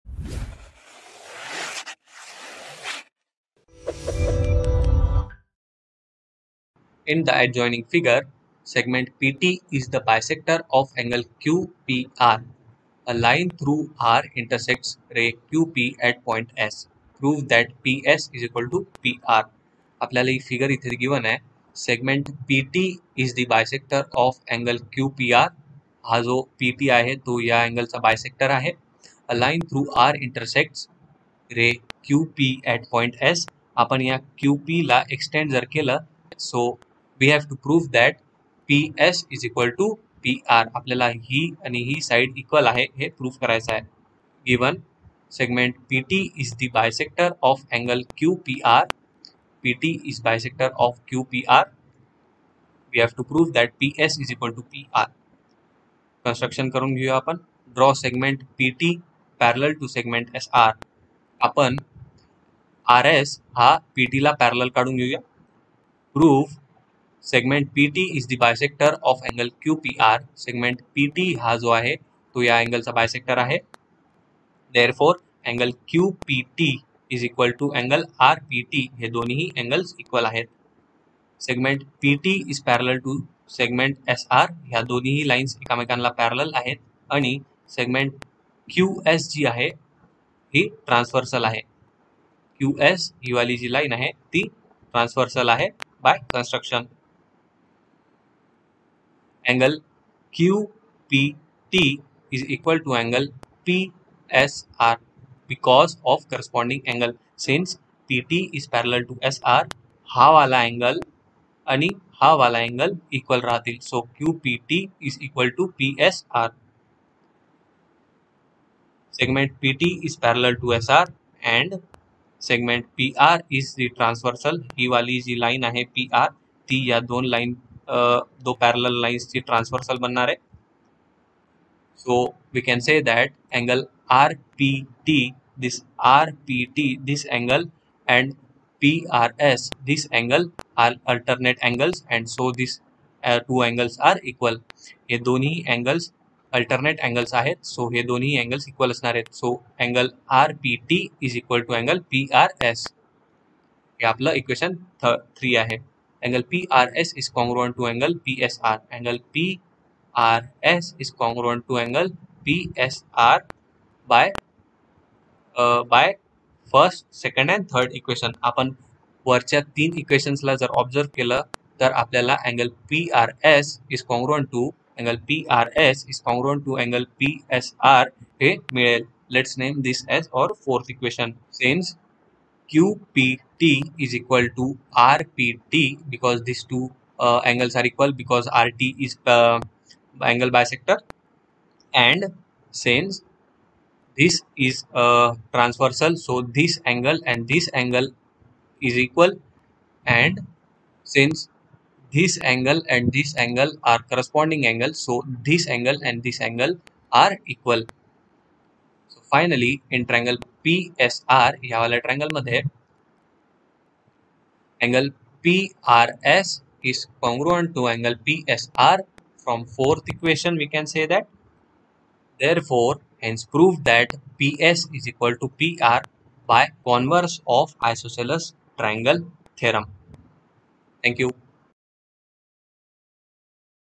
Solved numerical | Triangles | 9th SSC | Maharashtra Board | Alphy
In this video we are going to solve a numerical :

In figure, seg PT is the bisector of QPR. A line through R intersects ray QP at point S. Prove that PS = PR